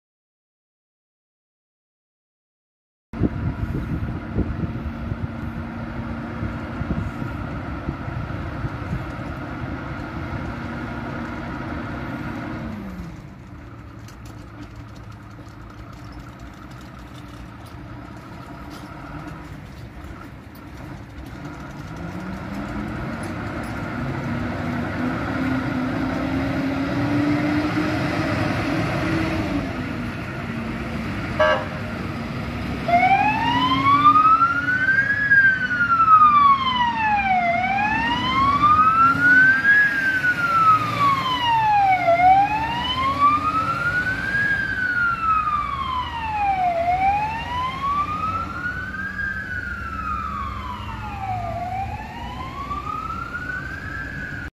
[ALARMOWO] Wyjazd alarmowo zastępu 301[T]25 do zdarzenia.
Zapraszam na mojego instagrama gdzie możecie oglądać zdjęcia tego oto wozu ale i innych pojazdów straży.